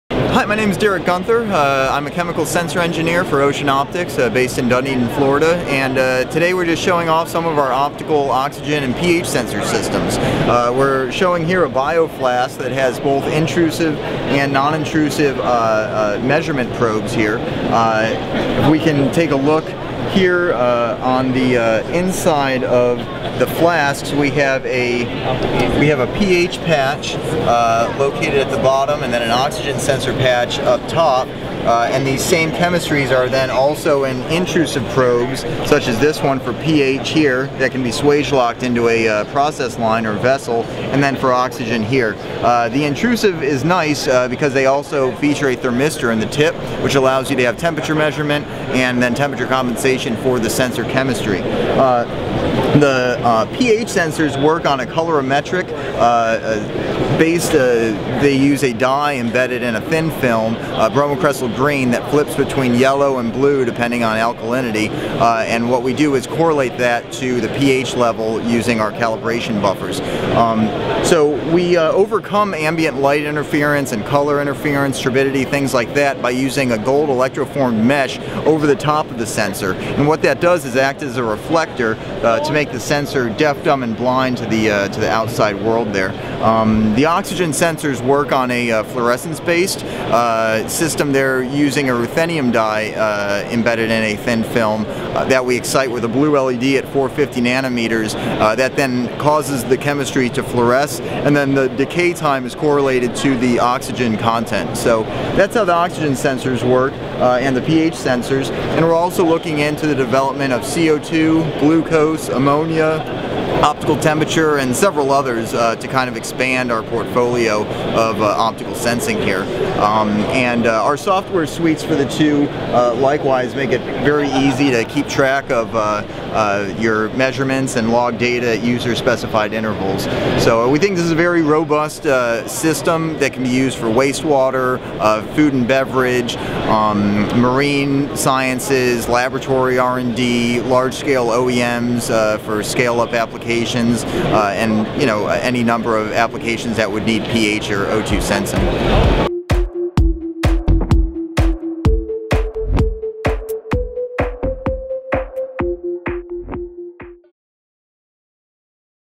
Optical Oxygen and pH Sensors Display by Ocean Optics
Derek Guenther discussed the optical sensing capabilities from Ocean Optics. for stopping in to shoot this video!